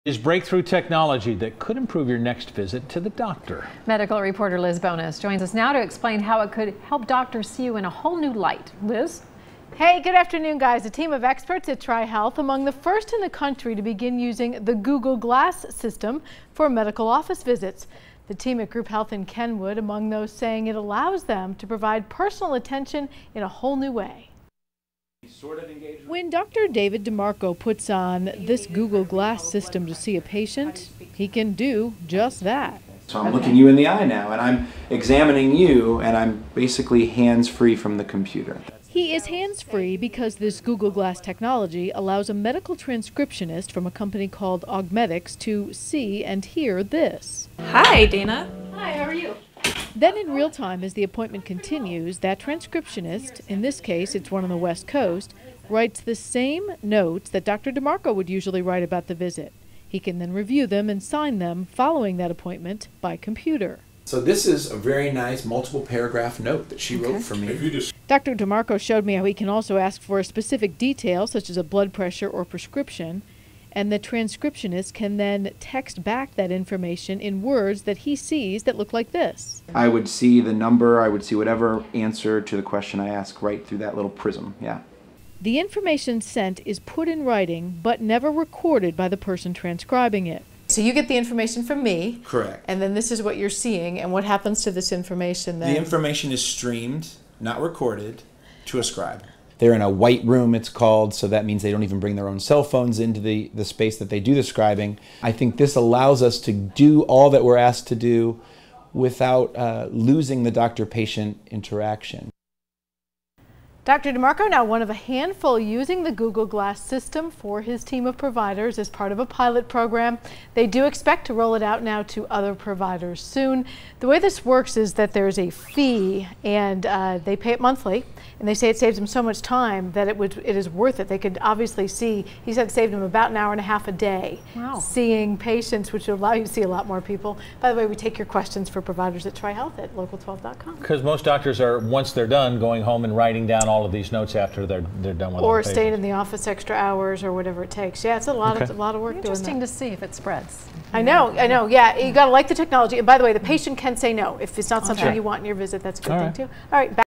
How does the Google Glass service work?
CINCINNATI (WKRC) - A team of experts at TriHealth were among the first in the country to begin using the Google Glass system for medical office visits.

The team at Group Health in Kenwood were among those saying it allowed them to provide personal attention in a whole new way. When Dr. David Demarco put on the glasses to see a patient, he can do just that.

"So I am looking you in the eye now and I am examining you and I am basically hands-free from the computer," said Dr. Demarco.

He was hands-free because the Google Glass technology allowed a medical transcriptionist from a company called Augmedix to see and hear the event in real time. As the appointment continues a medical transcriptionist, in this case on the West coast, writes the same notes that Dr. Demarco usually would write. He can then review and sign it following the visit by computer.

He can ask for a specific detail such as a previous blood pressure prescription detail and the transcriptionist can text that information back in words.

Dr. Demarco said, "I would see the number, I would see whatever the answer is to the question I ask right through that little prism, yeah."

The information sent is put in writing but the visit is not recorded by the person transcribing it, "They are in a white room, as it's called, so that means they don't even bring their own cell phones into the area where they do scribe," said Dr. Demarco.

Dr. Demarco os one of a handful now using the Google Glass system for his team of providers as part of a pilot program. They do expect to roll it out to other providers soon.

Health care systems negotiate a monthly fee which includes the technology and transcription. Patients can opt out if they don't want a provider to use it in a visit.